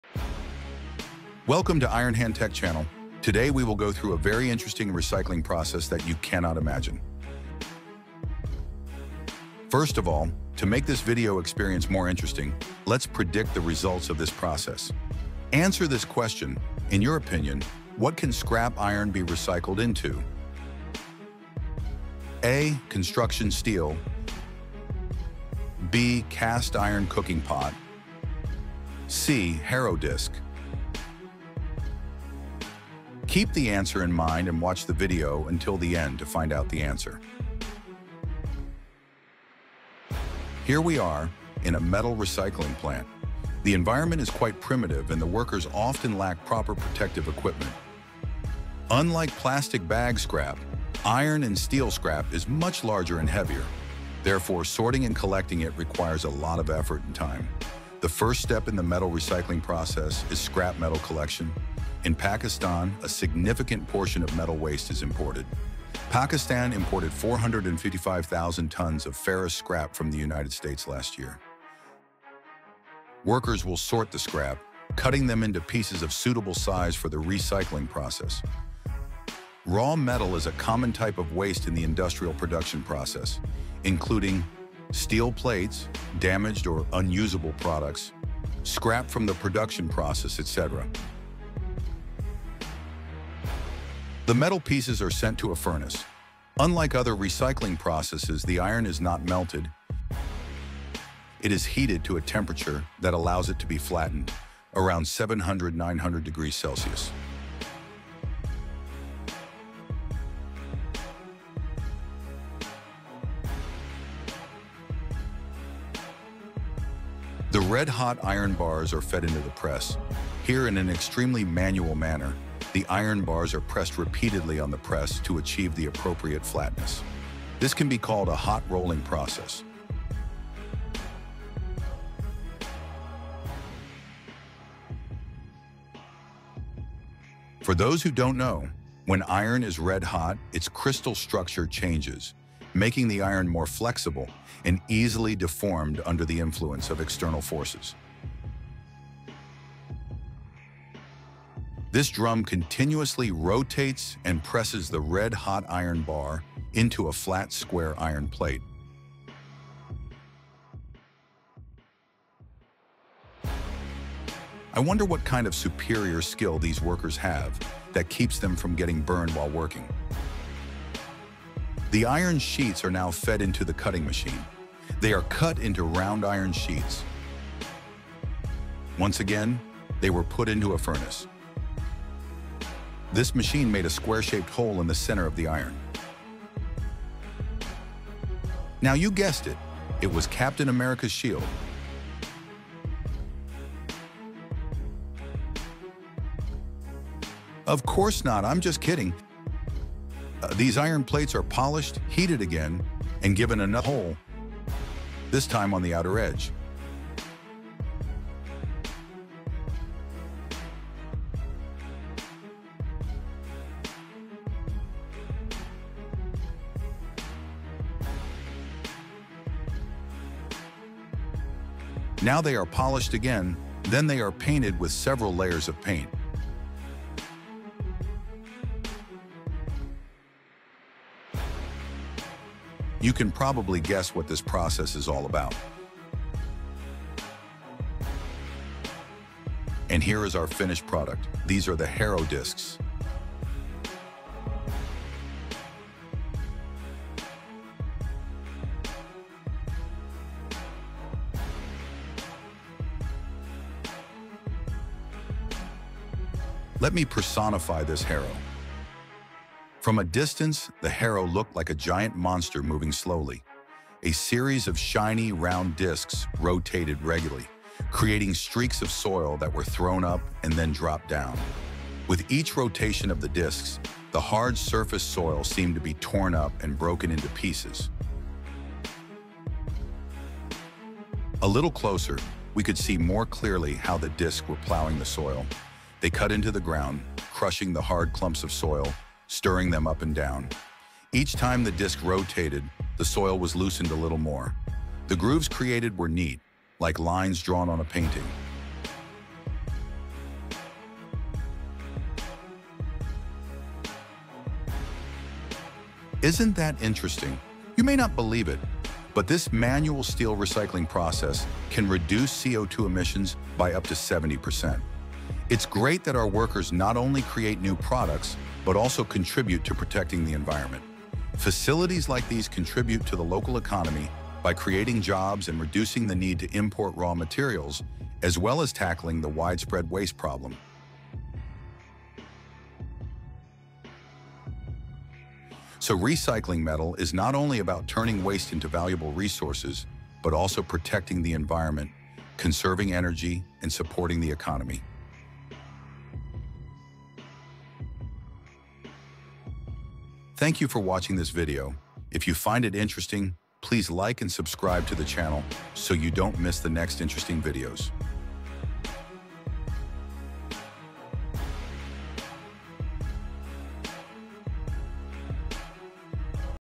Amazing process of Metal recycling - You will be surprised
The metal pieces are sent to a furnace. Unlike other recycling processes, the iron is not melted, it is heated to a temperature that allows it to be flattened, around 700-900 degrees Celsius.
LINK IronHand Tech:
We does not own the rights to these video clips.

✅ Thank you everyone for watching our video!